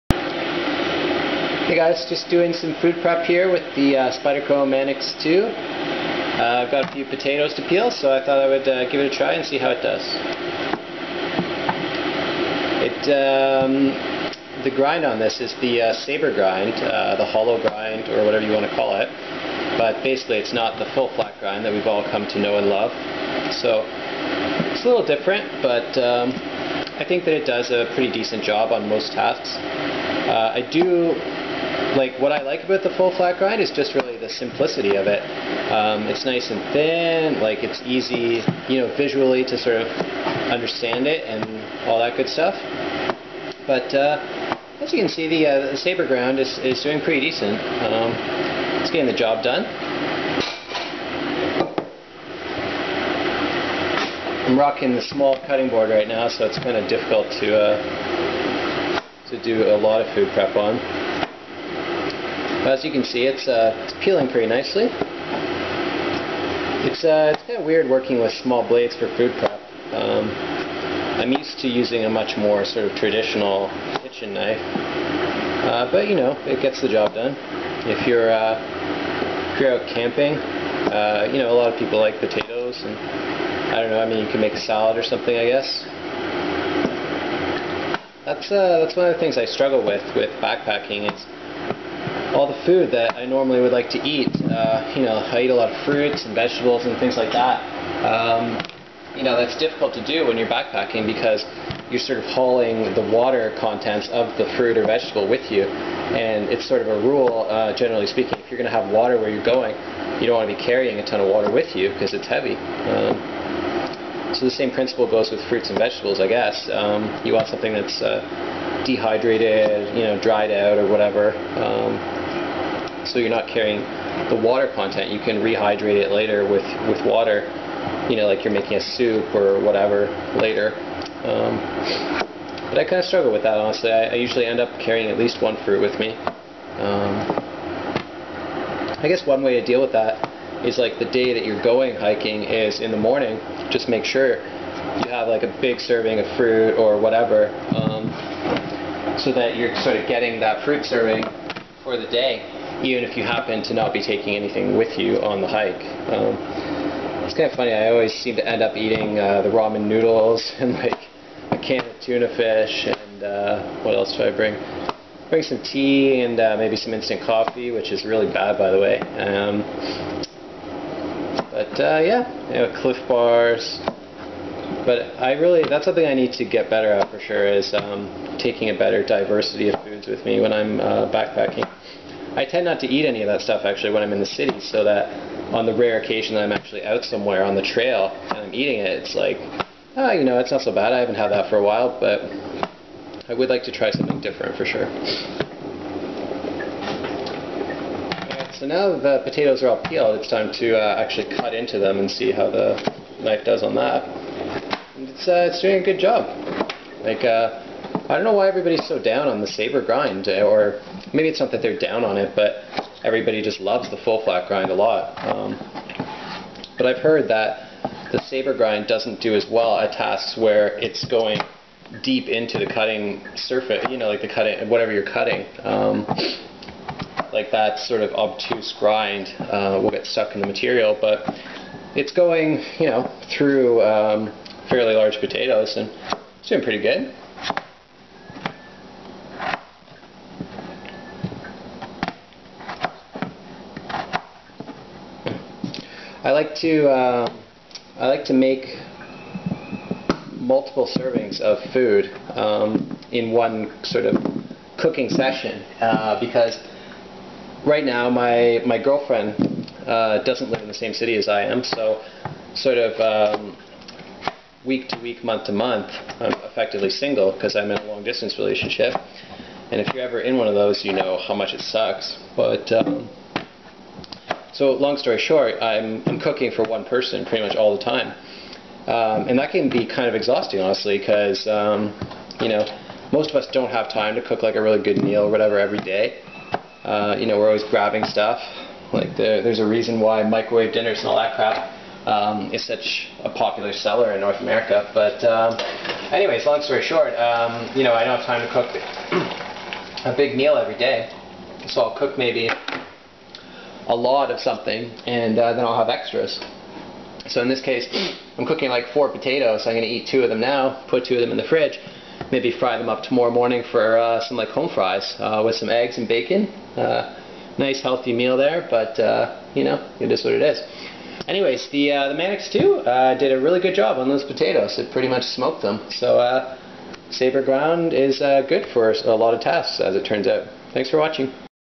Spyderco Manix 2 Potato Killer
Doing some quick food prep with the Spyderco Manix 2 s30v.  Also a brief discussion about backpacking food, blade grinds, and cooking.